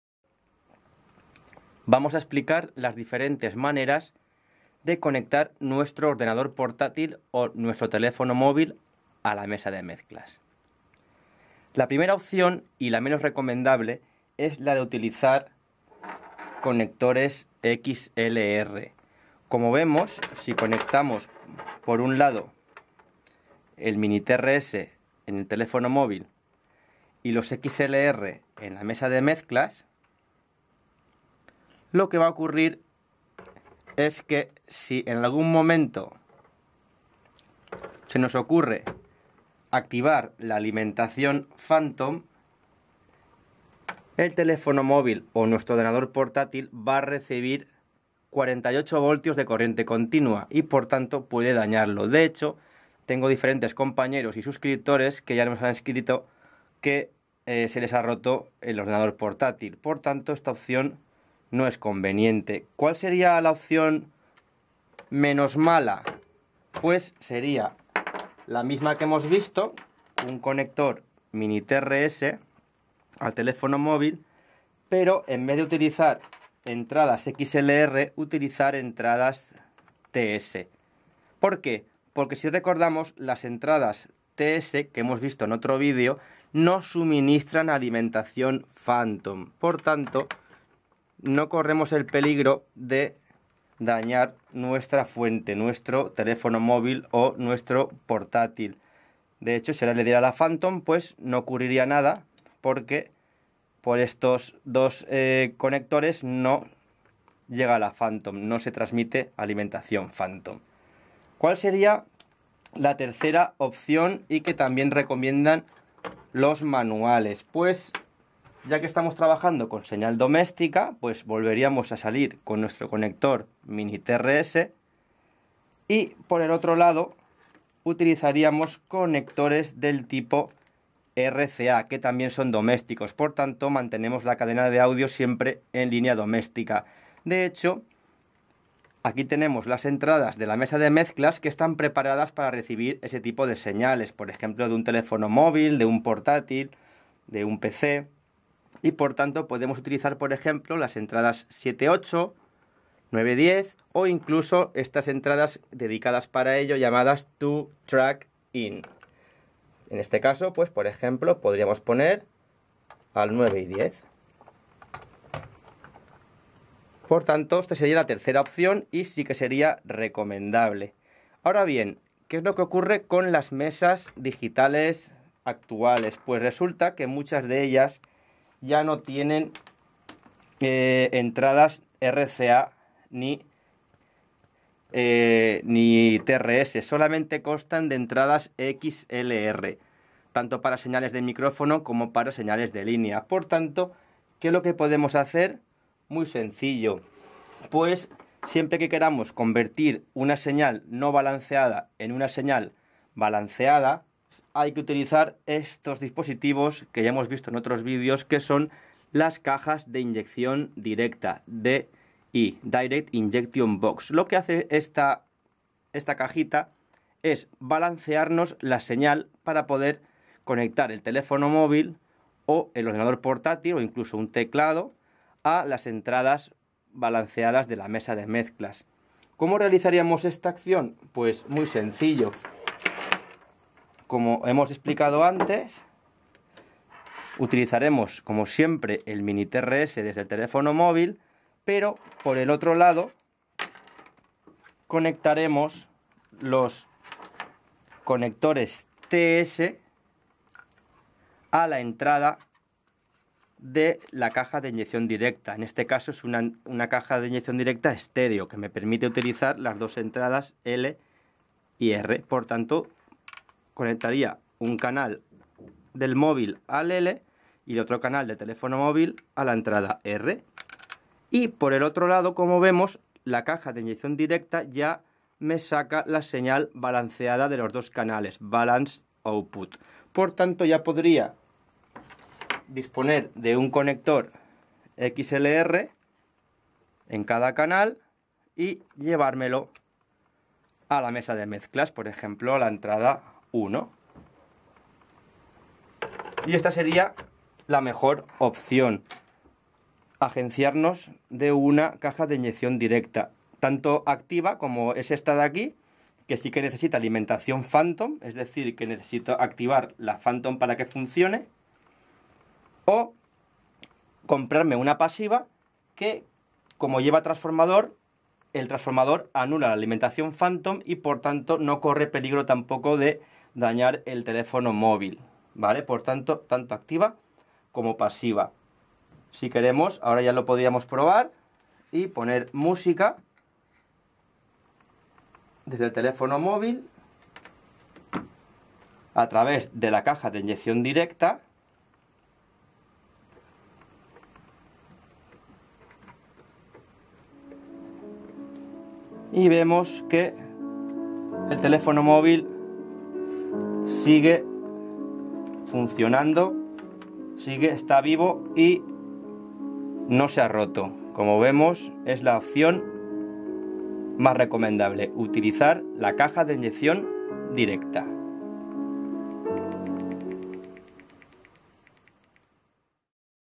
CÓMO CONECTAR EL ORDENADOR o TELÉFONO MÓVIL A LA CONSOLA SIN DAÑARLOS CON LA PHANTOM - NUNCA CON XLR
Este vídeo está dedicado a los técnicos de sonido, compañeros y curiosos que alguna vez han dañado su ordenador portátil o teléfono móvil por conectarlos a las entradas XLR-3 Cannon de la mesa de mezclas con la alimentación phantom activada. En este vídeo analizaremos cuatro maneras básicas de conectar el ordenador portátil o teléfono móvil a la mesa de mezclas:

1 - De MINITRS (MINIJACK) a DOS XLR-3 CANNON
NUNCA DEBEMOS HACERLO ASÍ, PORQUE SI ACTIVAMOS LA ALIENTACIÓN PHANTOM GENERAL DE LA MESA PARA LOS MICRÓFONOS DE CONDENSADOR, EL ORDENADOR O TELÉFONO RECIBIRÁ LOS 48 VOLTIOS DE CORRIENTE CONTINUA DE LA PHANTOM.

2 - De MINITRS a DOS TS - Mejor que la opción 1, ya que al menos no corremos peligro de dañar nuestro ordenador o teléfono, puesto que los conectores TRS de la mesa no suministran alimentación phantom.

3 - De MINITRS a DOS RCA - Es la opción más lógica porque mantenemos la cadena de audio dentro del ámbito doméstico. De hecho, incluso disponemos de entradas RCA llamadas 2-TRACK IN.

4 - CAJA DE INYECCIÓN DIRECTA para balancear la señal - Sin duda, la opción más profesional en cualquier evento o sonorización. Aunque es una opción cara, es muy fiable y más barata que la reparación del ordenador portátil o teléfono móvil.

La mesa de mezclas del vídeo está descatalogada. Un modelo nuevo similar es el YAMAHA MG10XU

Vídeo realizado en el IES La Marxadella. Gracias a mi compañera Maribel Hervás por su colaboración durante la grabación.

Publicaciones del mismo autor del vídeo: EL LINE ARRAY

LIBRO EL LINE ARRAY EN FORMATO PAPEL:
Amazon Europa:

Amazon Reino Unido:

Amazon América:

Amazon México